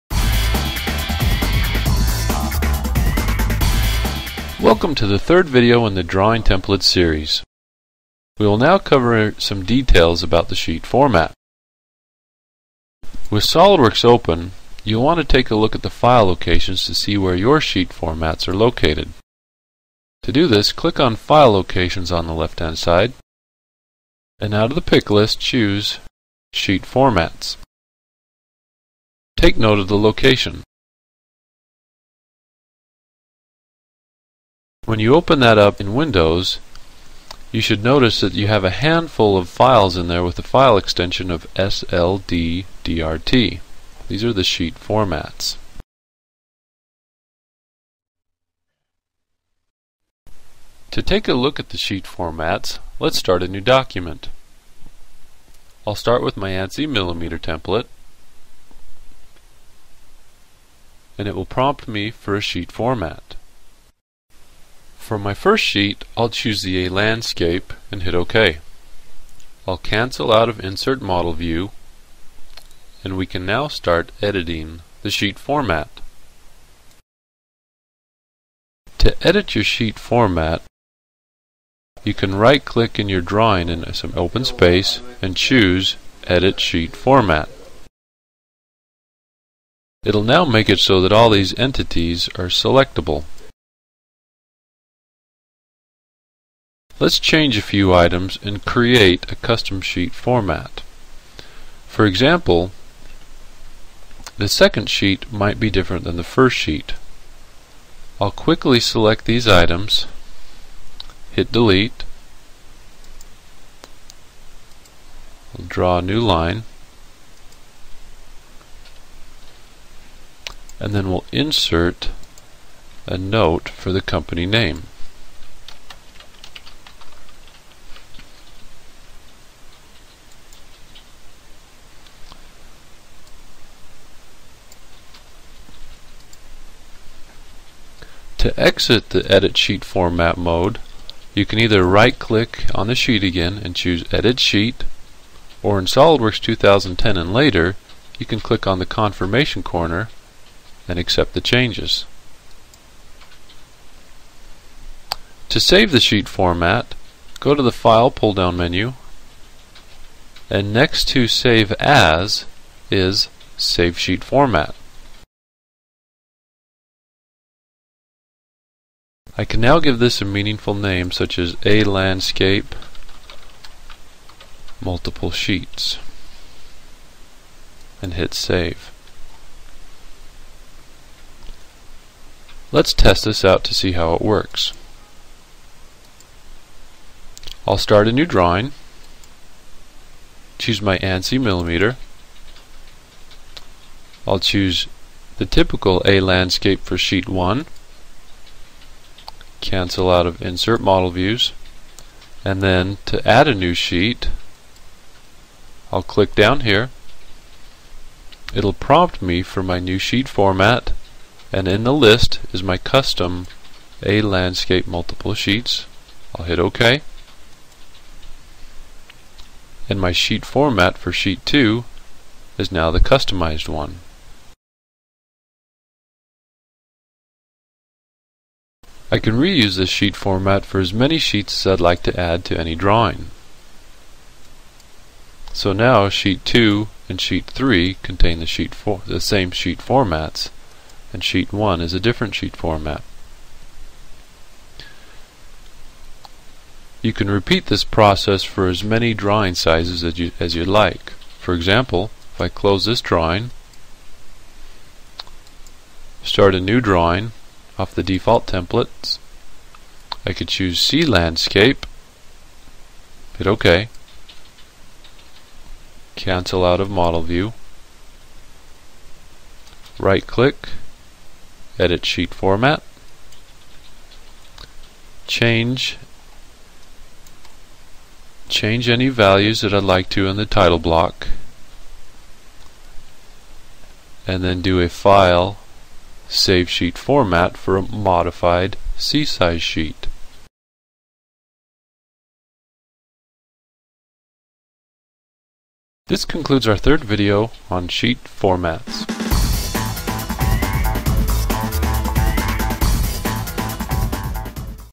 SOLIDWORKS - Drawing Templates Pt. 3 of 3
Learn how to edit sheet formats in this final video of a three part series on Drawing Templates in SOLIDWORKS 2010.